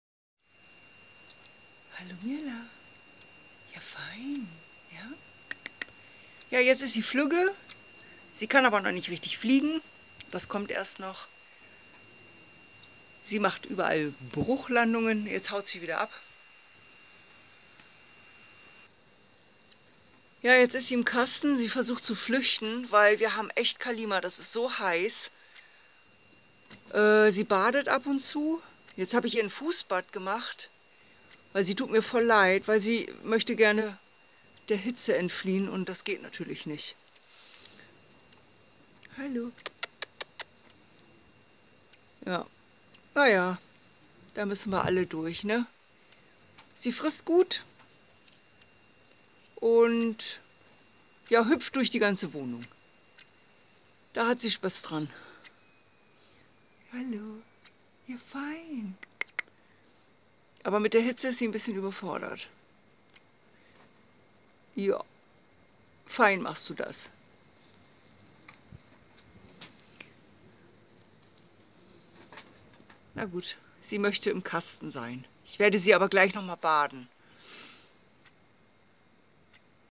mirla und die hitze
nachts hatten wir 41 grad...für meine tiere eine fürchterliche hitze